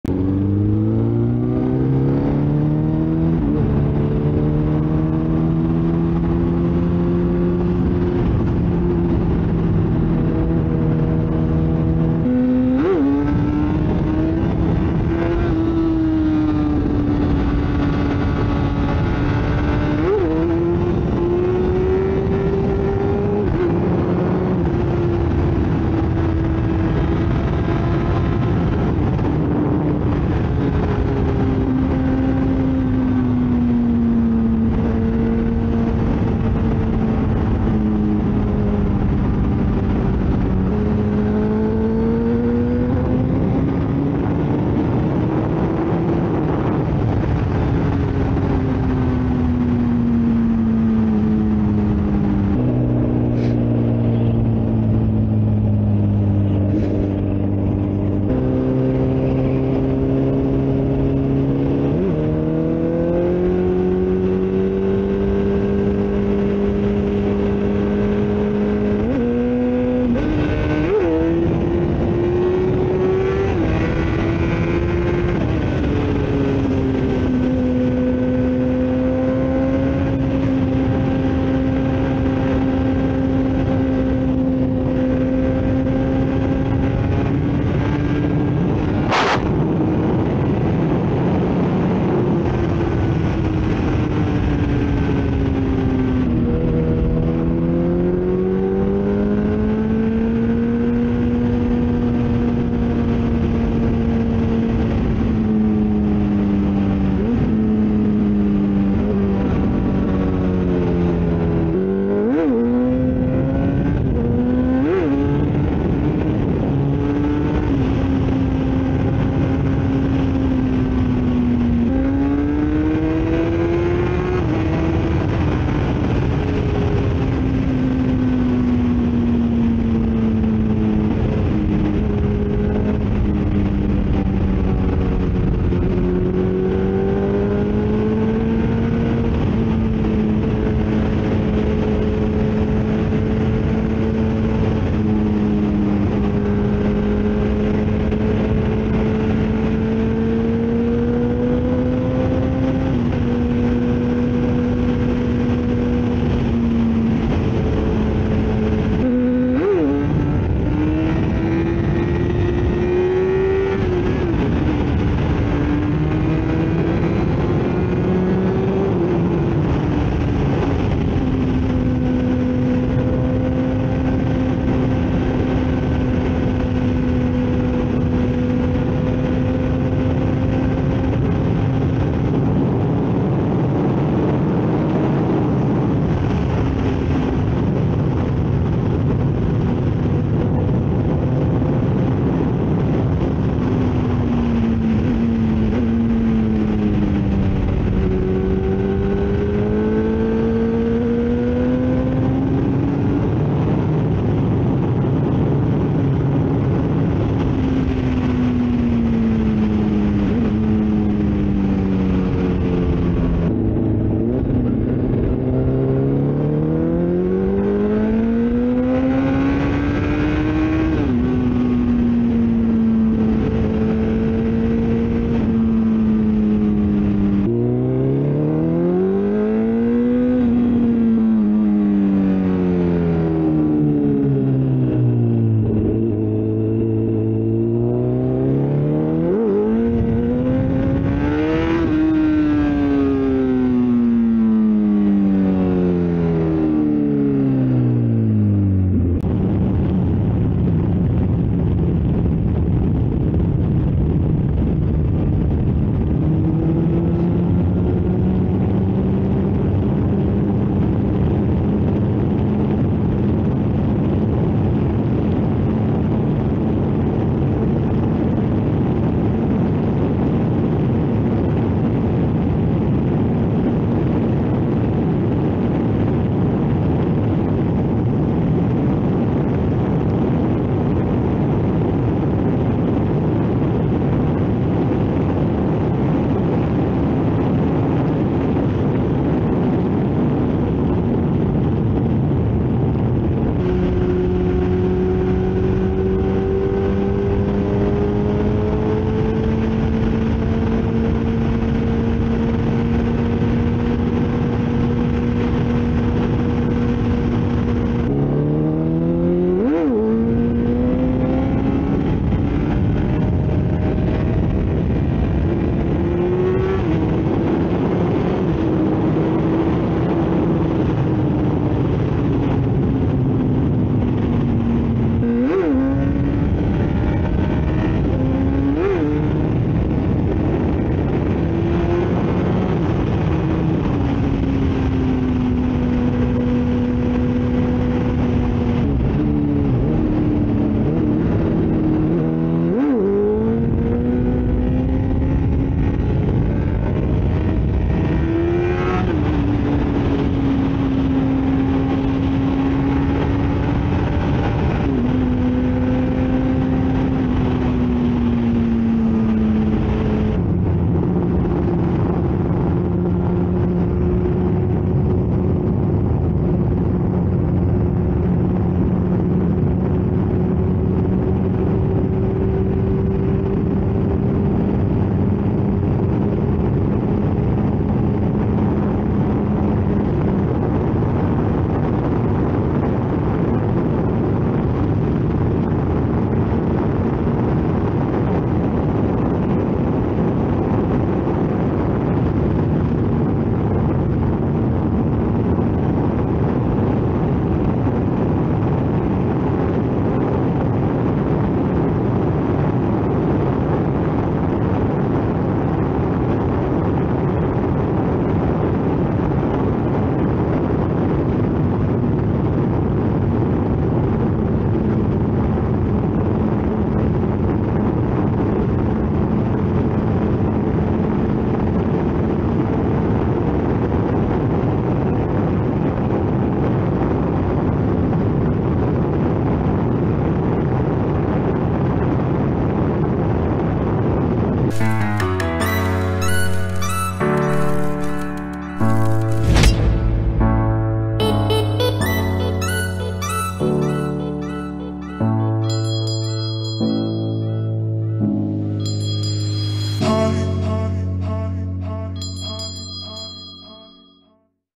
CBR 1000RR FireBlade is back!! Crazy night ride! 4K
You were missing the fireblade videos? So, here it is! Watch in 4k and tell me what you think of the image quality!

👉 My bikes
🏍 Honda CBR 1000RR Fireblade 2004 sc57.
🏍 Honda CBR 600 F4i 2001
🏍 Honda MSX (Grom) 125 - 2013